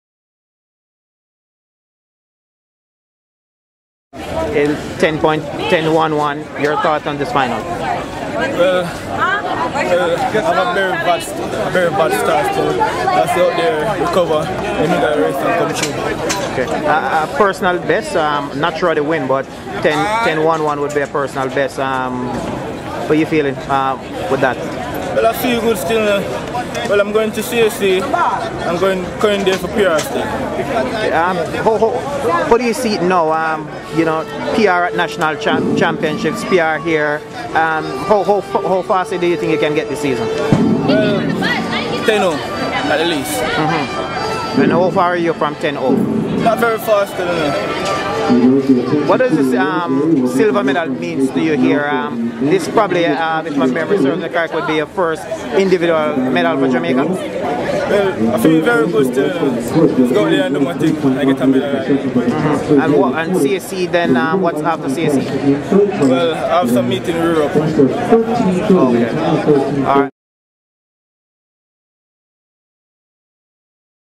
Oshane Bailey interview after his 10.11 PB
Oshane Bailey speaks after his personal best 10.11 seconds run at the NACAC Under-23 Champ[ionships in Miramar, Florida.

Bailey, a former Calabar Student, had ran a then personal best of 10.14 to win the National Senior Trials.